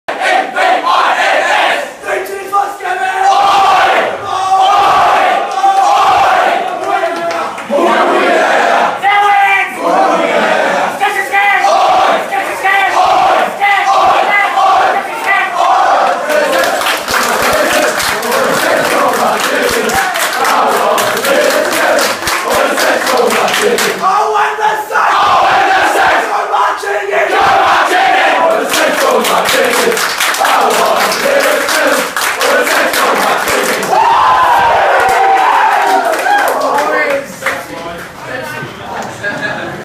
St Kevin's sing the song
After winning the last game of the final season against Caulfield